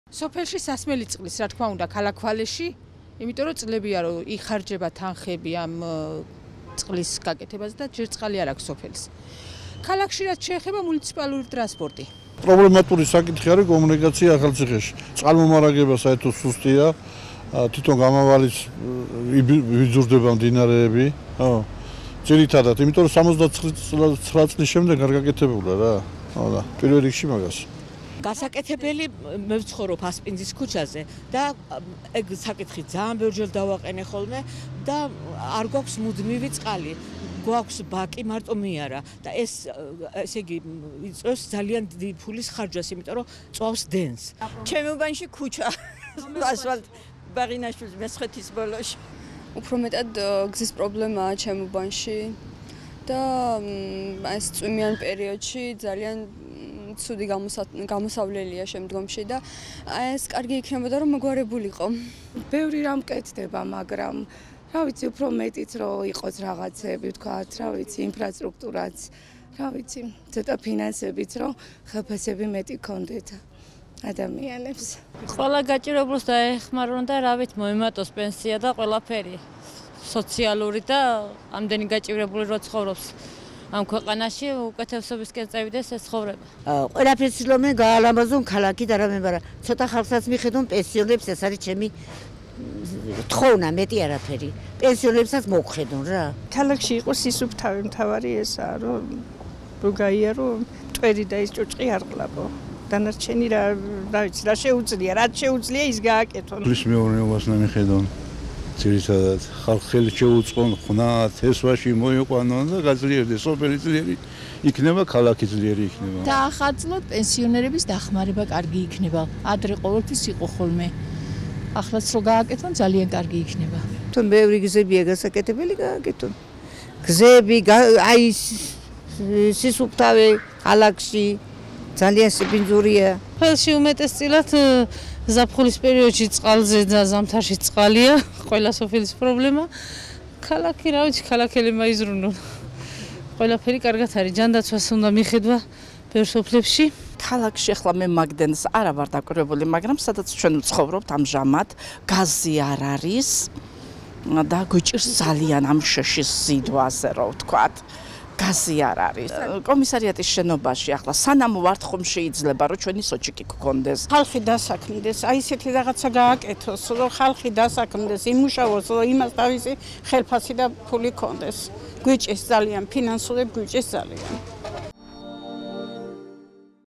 გამოკითხვა: რა პრობლემების მოგვარება სურთ ახალციხელებს 2023 წლის ბიუჯეტით
ახალციხის მუნიციპალიტეტის საკრებულო უახლოეს დღეებში 2023 წლის ბიუჯეტს დაამტკიცებს, ნახეთ გამოკითხვა რა პრობლემების მოგვარება სურთ ახალციხის მუნიციპალიტეტში მცხოვრებლებს და  რა მიაჩნიათ  2023 წლის ბიუჯეტში გასათვალისწინებელ პრიორიტეტებად.